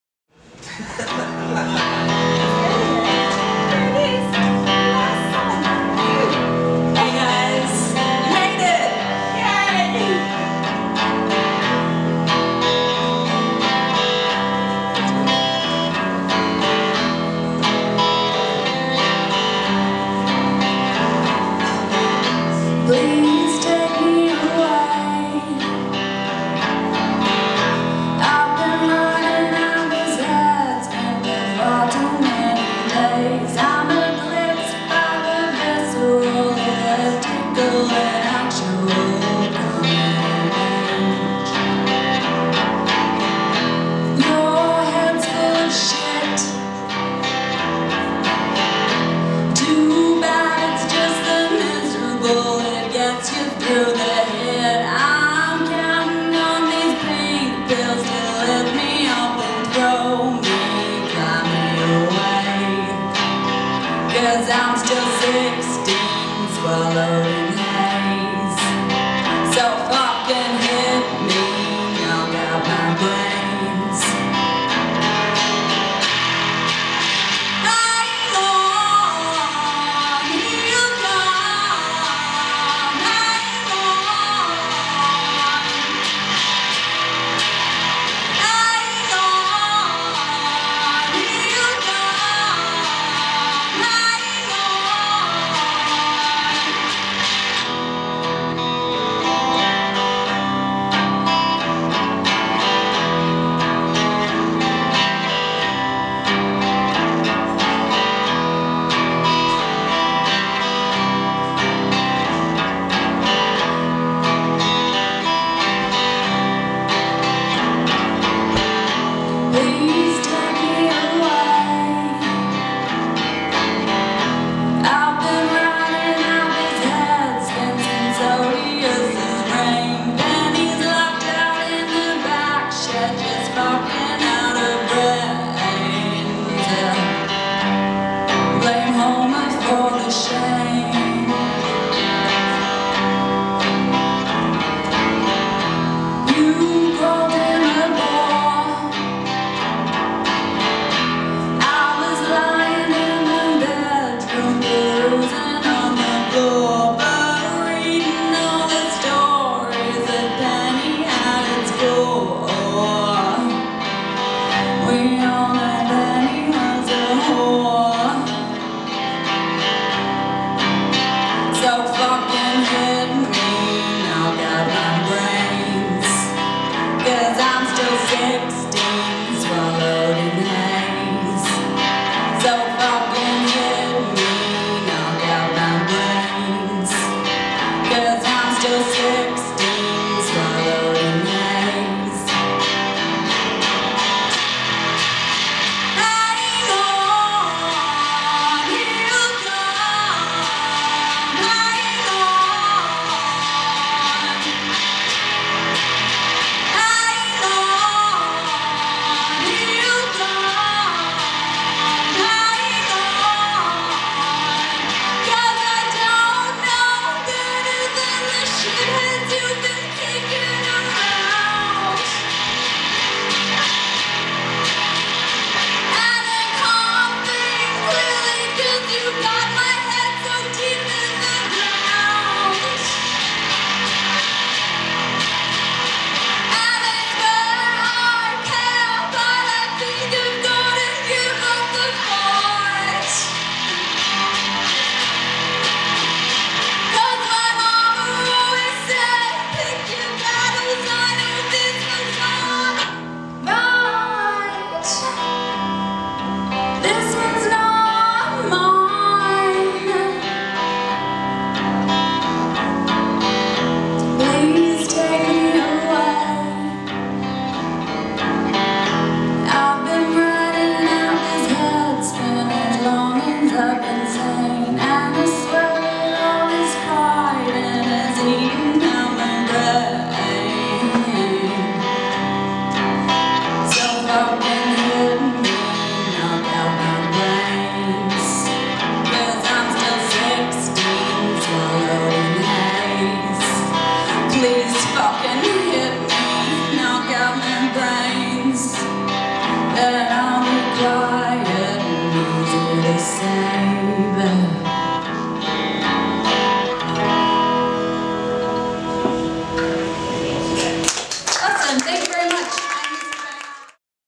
Oosterbaanger - Beaufort St Songwriters Club - Aug 18
Artist: OOSTERBAANGER
Live at the Beaufort St Songwriters Club
Thursday August 18, 2016

Beaufort St Songwriters Club happens every Thursday in Defectors bar above the Flying Scotsman in Mt Lawley.